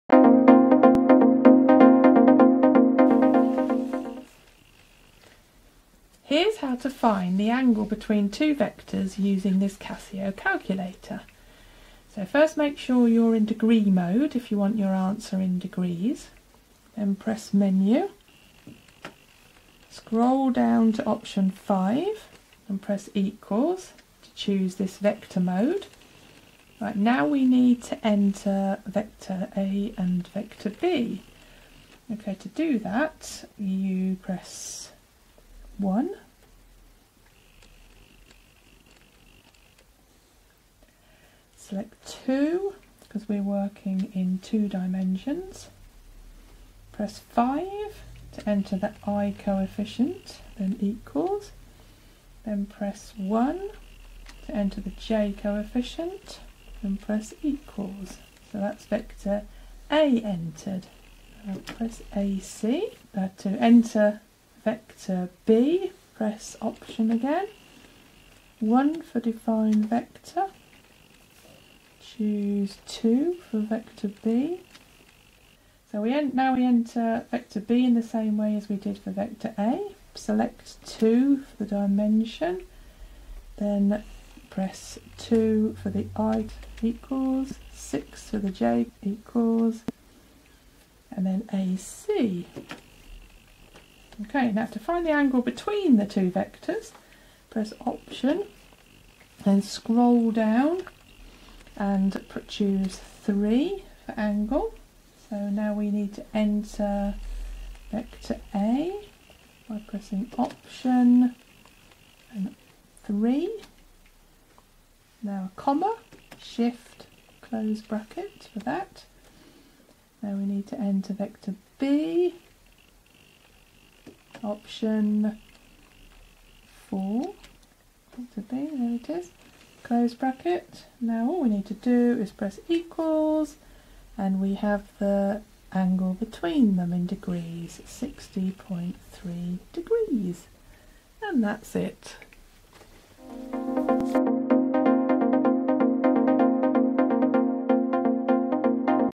Find the Angle Between Two Vectors on the Casio FX-991EX Classwiz Calculator
Scientific Calculator Tutorial. AS Maths - How to use a Casio FX-991EX Classwiz calculator to find the angle in degrees between a pair of 2D vectors written in Cartesian [i,j] form.